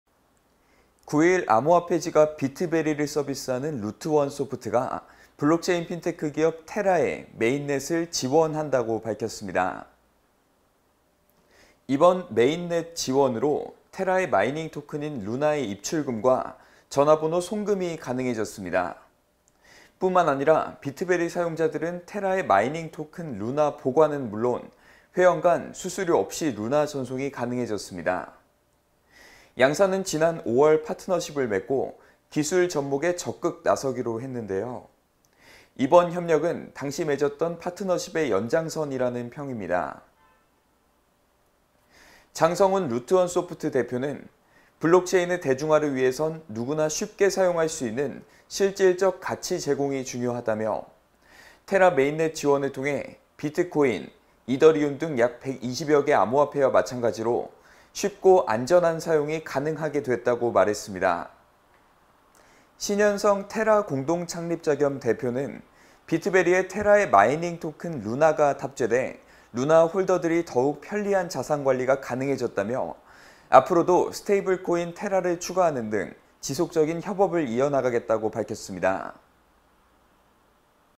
[라이브썰] ‘비트베리’ 운영사 루트원소프트, “테라 메인넷 지원”
9일 암호화폐 지갑 ‘비트베리’를 서비스하는 루트원소프트가 블록체인 핀테크 기업 ‘테라’의 메인넷을 지원한다고 밝혔습니다.

이번 메인넷 지원으로 테라의 마이닝 토큰인 루나의 입출금과 전화번호 송금이 가능해졌습니다. 뿐만 아니라 비트베리 사용자들은 테라의 마이닝 토큰 루나 보관은 물론 회원 간 수수료 없이 루나 전송이 가능해졌습니다. 양사는 지난 5월 파트너십을 맺고 기술 접목에 적극 나서기로 했는데요. 이번 협력은 당시 맺었던 파트너십의 연장선이라는 평입니다.

장성훈 루트원소프트 대표는 “블록체인의 대중화를 위해선 누구나 쉽게 사용할 수 있는 실질적 가치 제공이 중요하다”며 “테라 메인넷 지원을 통해 비트코인, 이더리움 등 약 120여개 암호화폐와 마찬가지로 쉽고 안전한 사용이 가능하게 됐다”고 말했습니다.

신현성 테라 공동 창립자 겸 대표는 “비트베리에 테라의 마이닝 토큰 루나가 탑재돼 루나 홀더들이 더욱 편리한 자산 관리가 가능해졌다”며 “앞으로도 스테이블 코인 테라 추가 등 다방면의 협업을 이어나갈 것”이라고 말했습니다.